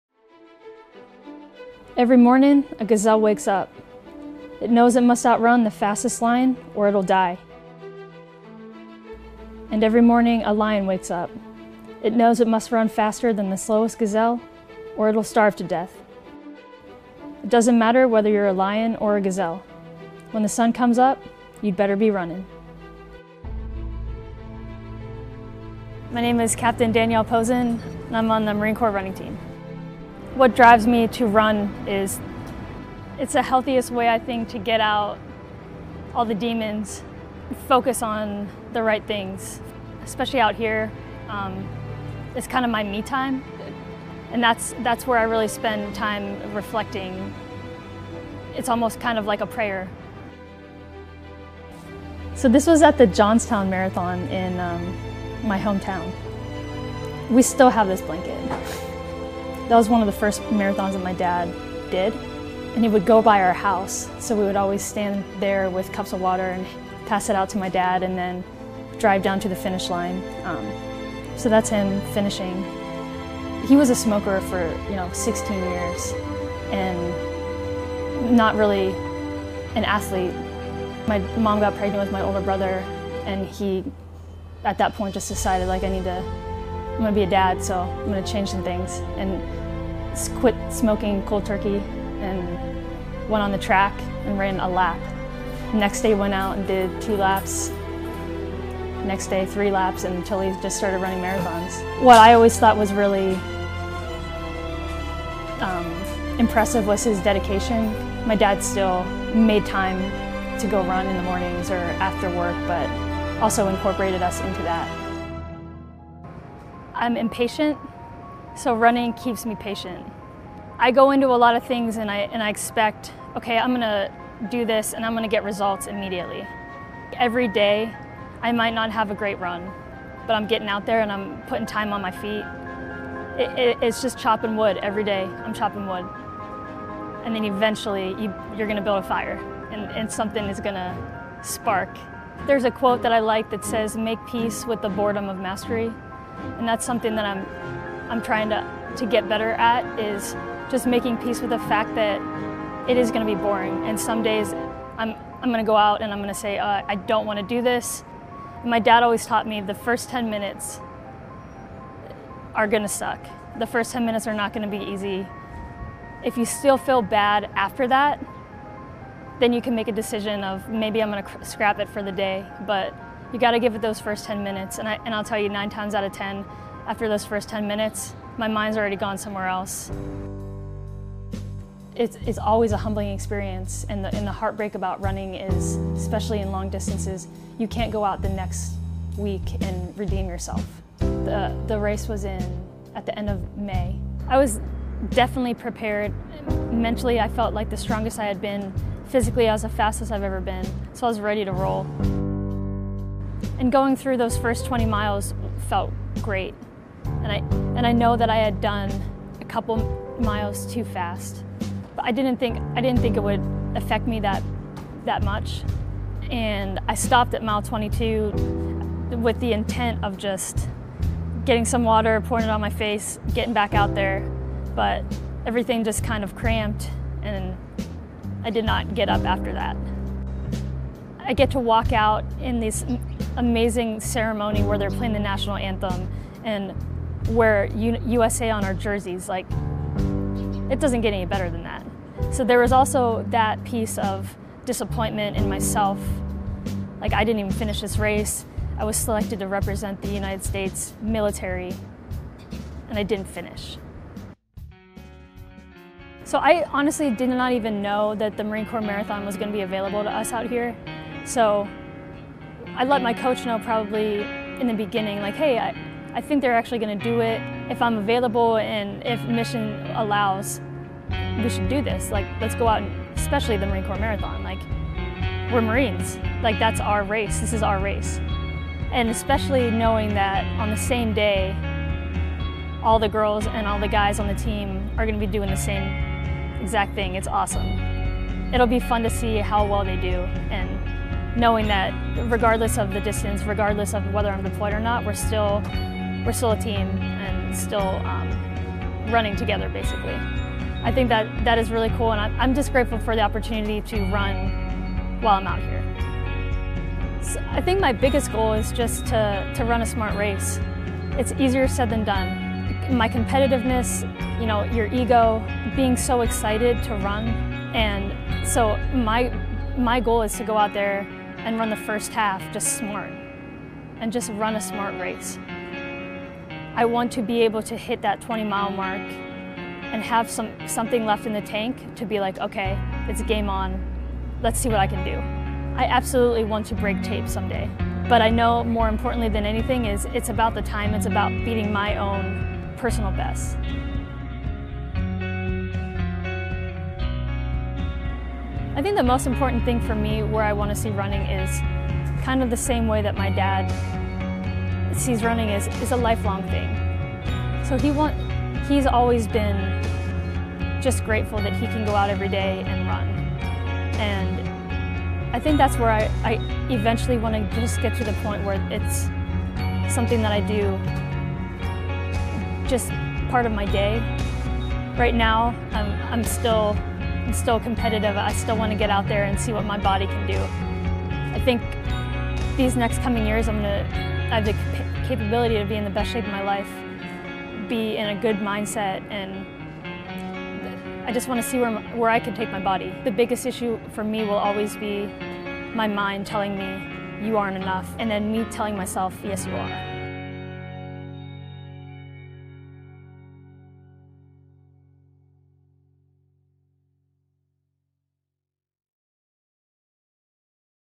Marines | You better be Running
Captain Danielle Pozun, a runner on the Marine Corps Running Team, is deployed with Special Purpose MAGTF - Crisis Response - Central Command in the U.S. Central Command area of operations.

She ran the 42nd Marine Corps Marathon Forward Oct. 22, 2017. Here is her story.

(U.S. Marine Corps video by Sgt. Conner Robbins and Staff Sgt. Phillip Elgie)